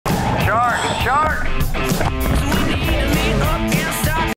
SHARK BITE!
Gotta keep your eyes peeled when you have a stringer full of fish!